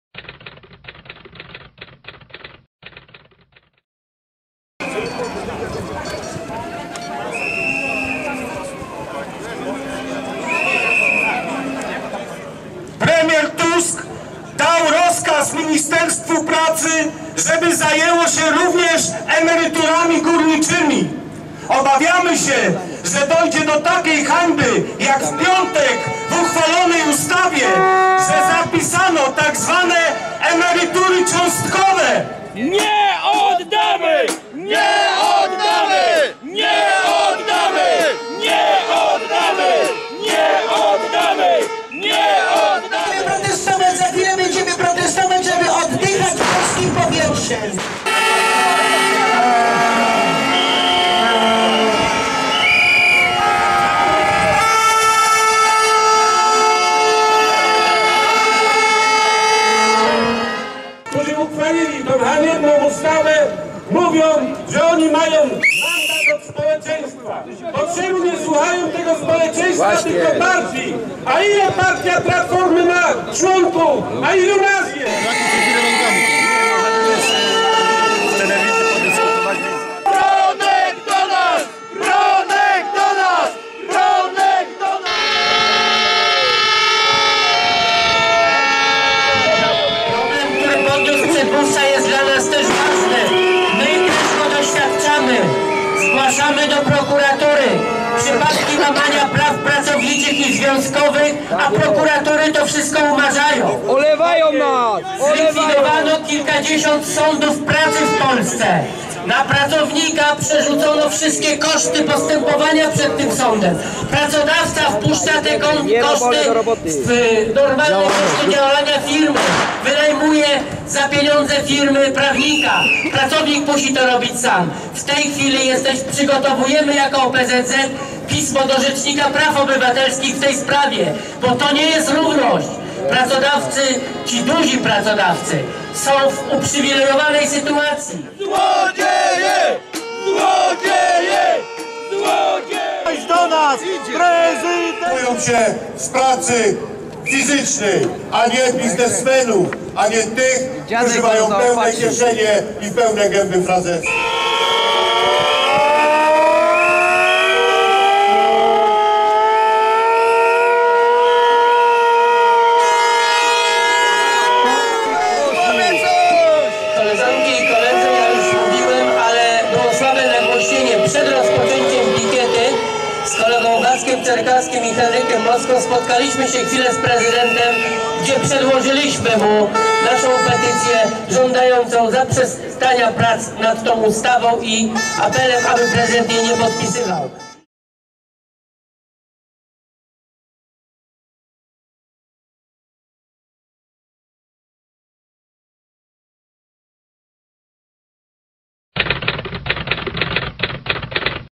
Manifestacja w Katowicach
Manifestacja w Katowicach przeciwko podniesieniu wieku
emerytalnego.
Wręczenie petycji prezydentowi Komorowskiemu